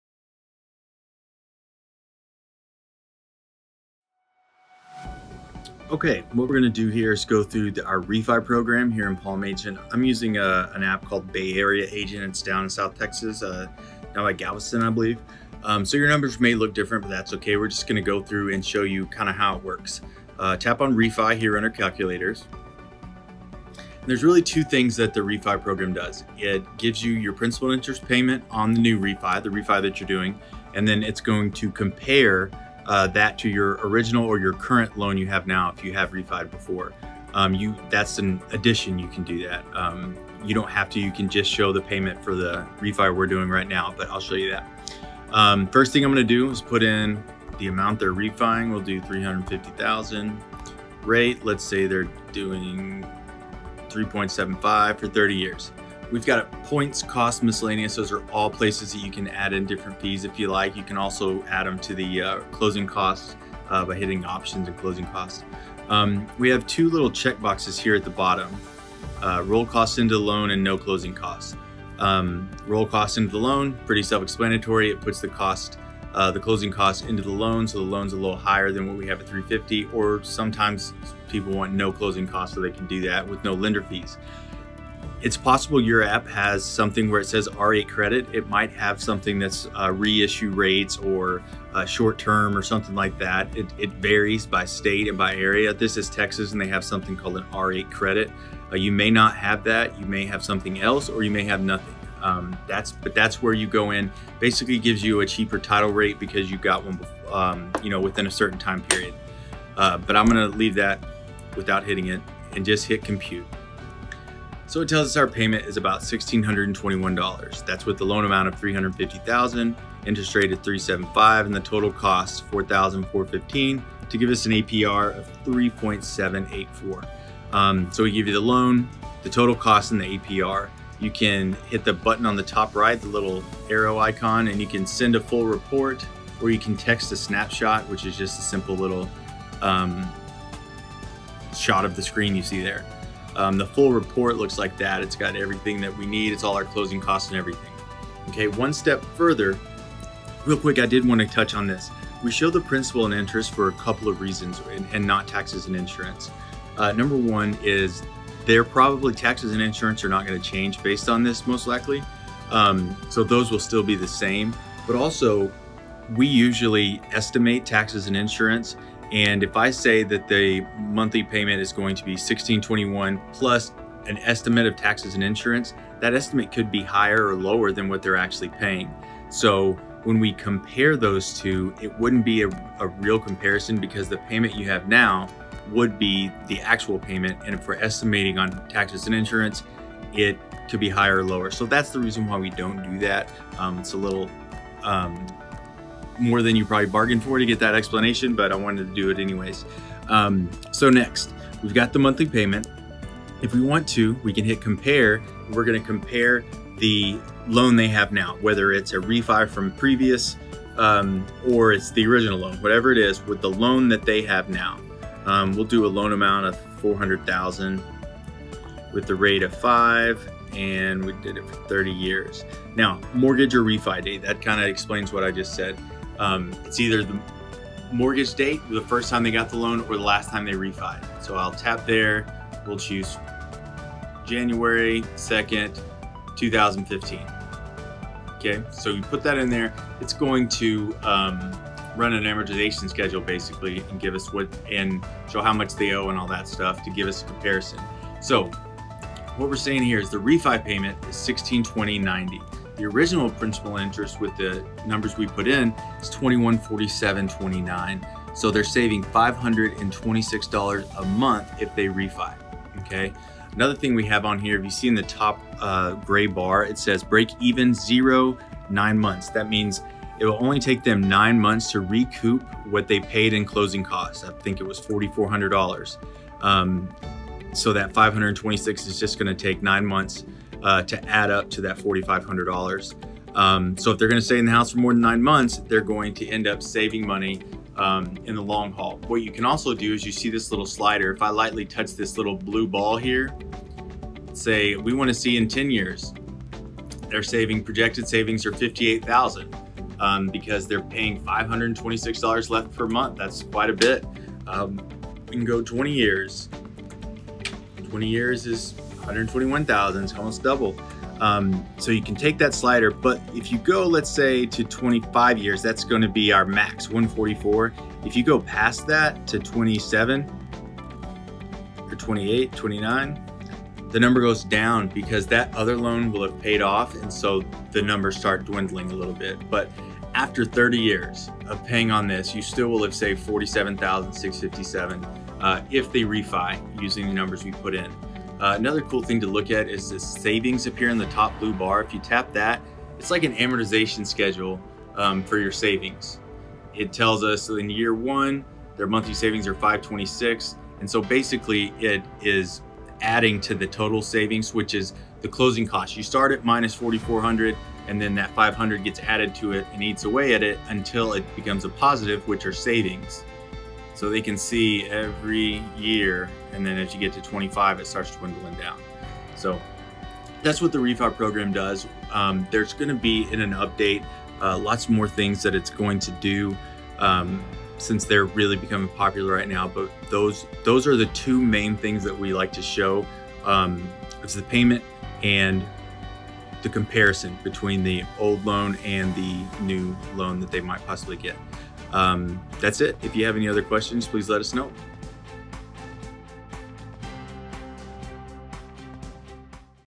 REFI Calculator Tutorial
Learn how to run refinance scenarios with your app quickly and easily!